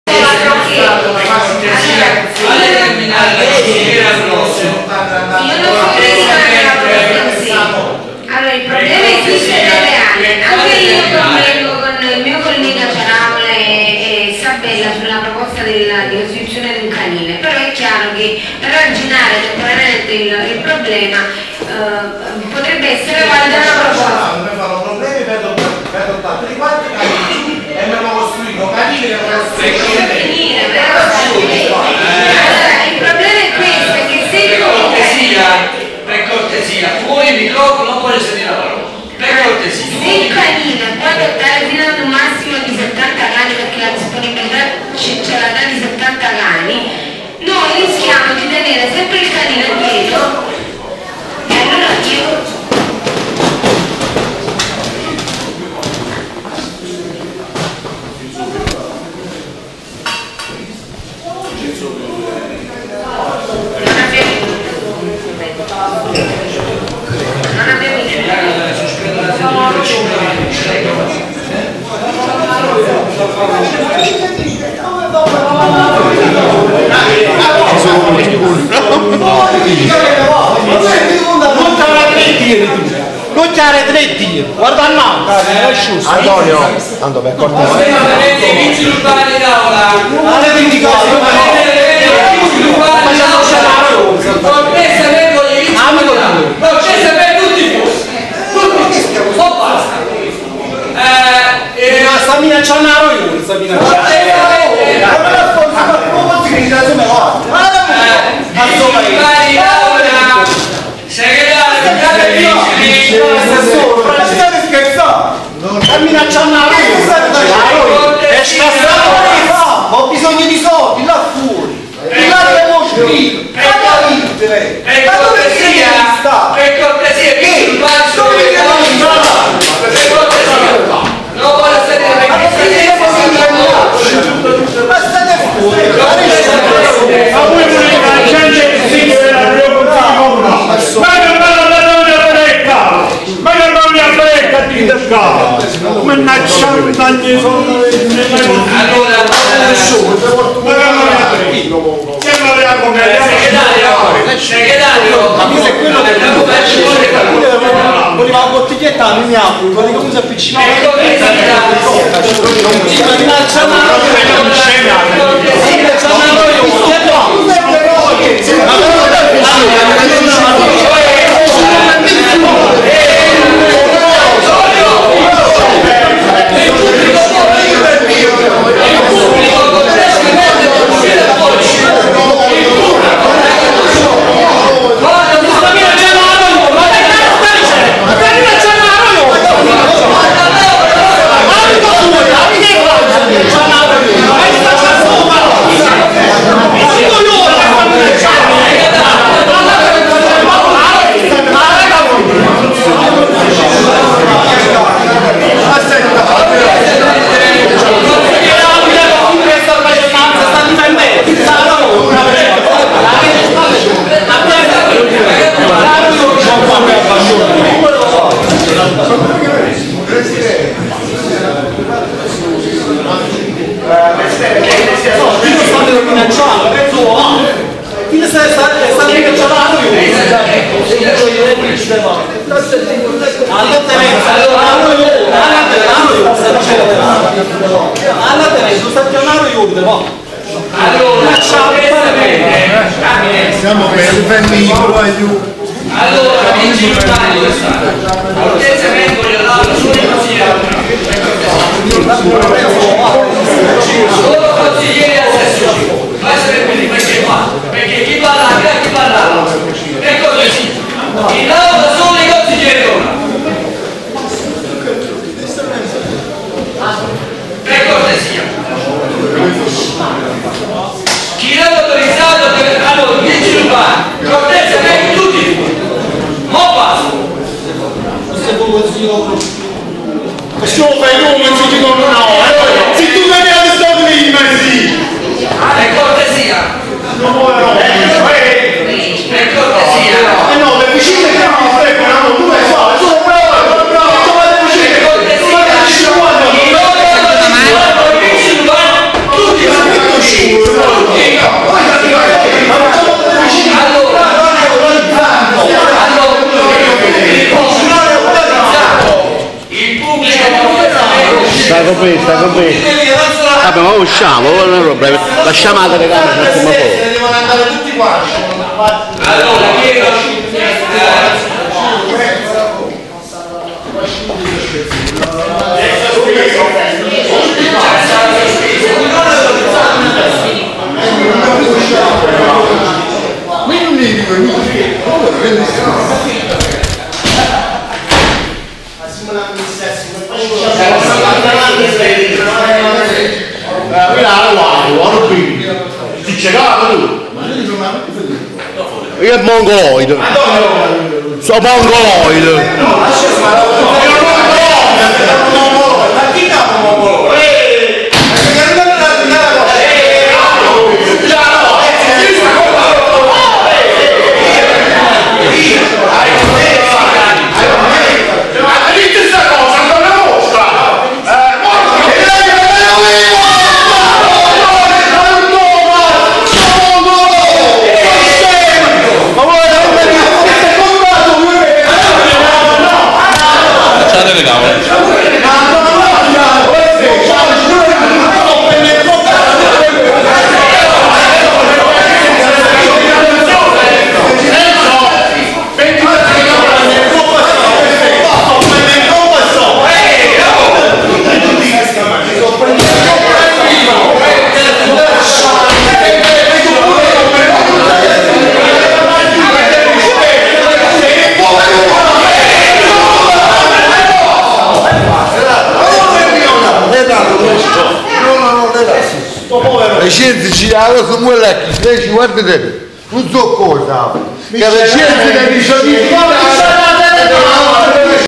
Terzigno: Consiglio Comunale o............................
Quello che resta di 6 ore di Consiglio Comunale finito in caciara, ma basta ed avanza per evidenziare in modo netto che ai Consiglieri Comunali la telecamera degli Amici di Beppe Grillo di Terzigno , che mette a conoscenza la cittadinanza della vita amministrativa non fa piacere, poichè mette a nudo la loro vera essenza e capacità amministrativa. Da notare il "meschino" nell' apparire disinvolto Antonio Vaiano che come cieco si avventa col suo delicato corpo sulla telecamera, al fine di sbatterla per terra o almeno girarla verso il muro. VERGOGNOSO come se non bastasse hanno voluto che venisse allontanata persino la VIDEOCAMERA. ! CERCHEREMO SE POSSIBILE PROVARE A RECUPERARE IL RESTO DEL FILMATO.